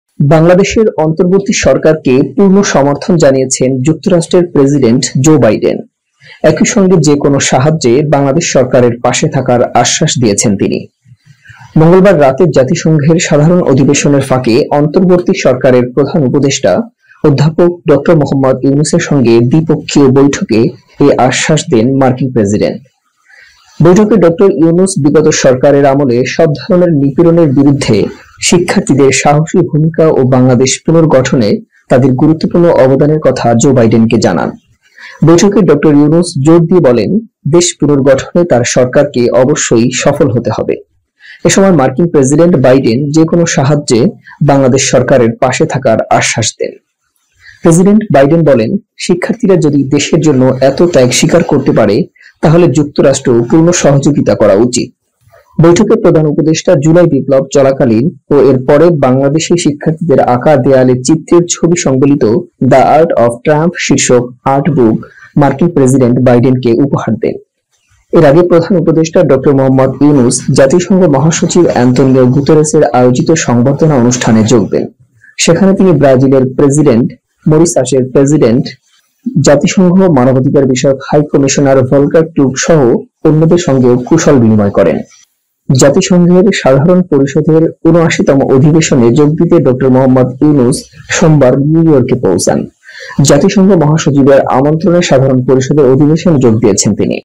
ড. ইউনূসের সঙ্গে বৈঠকে যে আশ্বাস দিলেন বাইডেন | Ajker bangla khobor | Dr Yunus | Joe biden
এইমাত্র পাওয়া Ajker Khobor 25 September 2024 | Bangladesh news today | Ajker bangla khobor